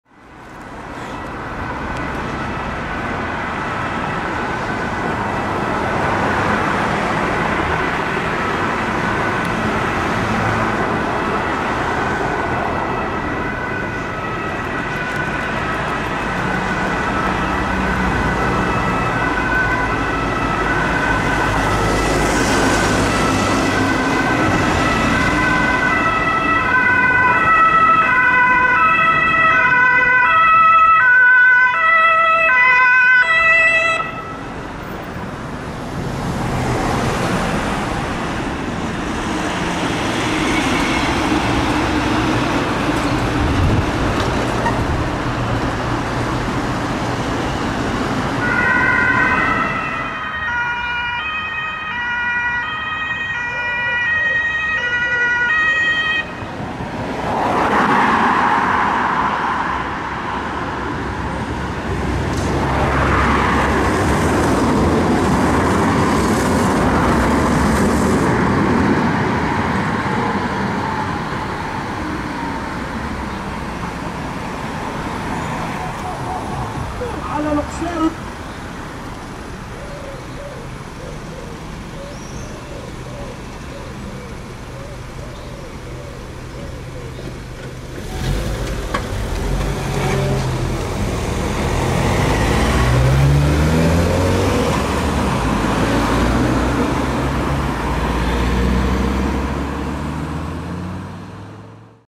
RTW 71-83-1 und RTW 14-83-2 DRK Rettungsdienstverbund Anhalt-Bitterfeld
RTW 71-83-1 der Rettungwache Radegast und der RTW 14-83-2 der Rettungwache Bobbau. Der RTW aus Radegast verfügt über eine Hänsch Tonfolgeanlage 520, aber es könnte eine ausländische Version sein. Desweiteren ist der Radegaster auch als S-RTW (Schwerlast-Rettungswagen) einsetzbar. Beide fahren im Status 7 jeweils in zwei unterschiedliche Krankenhäuser.
Mittlerweile gibt es neue RTWs von der Firma Strobel.

Stadt/Ort: Halle (Saale) | Sachsen-Anhalt | Deutschland/Germany |
02-05/2020

© Einsatzfahrten Sachsen-Anhalt